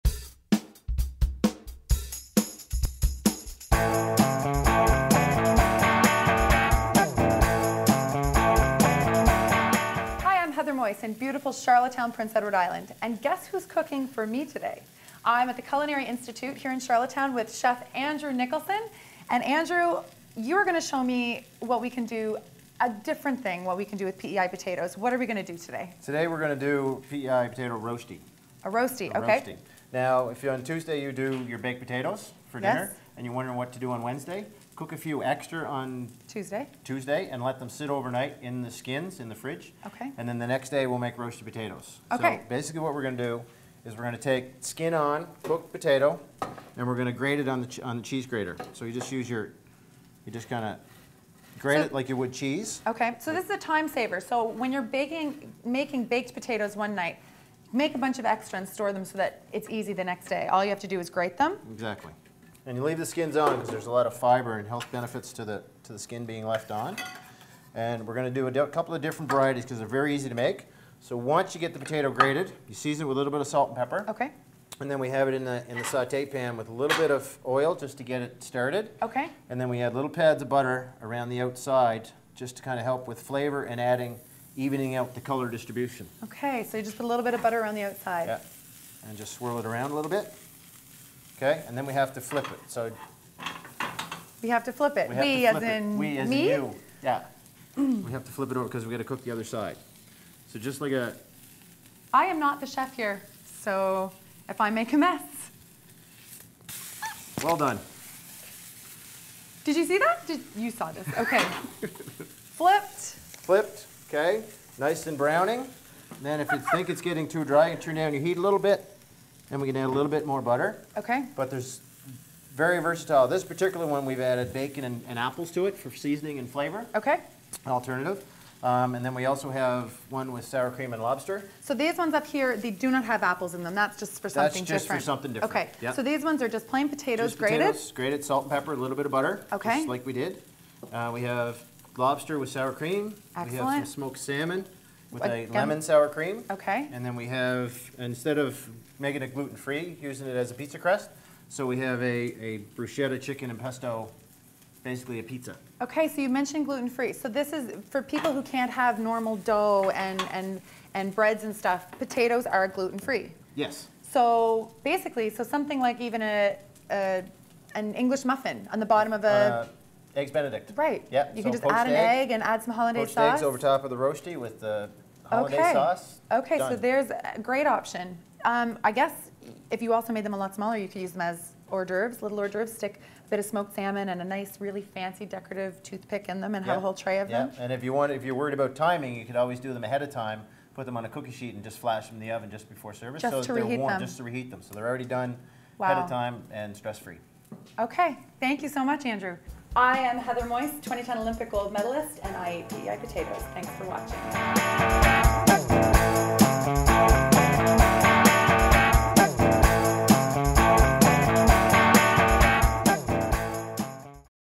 Cooking PEI Potatoes With Heather Moyse - Episode 2
This is the Second video in a series of three, where Chef Andrew Nicholson shows 2010 Olympic Gold Medalist Heather Moyse different ways to cook PEI Potatoes.

In this Episode we are; cooking some PEI Potato Rosti... Delicious!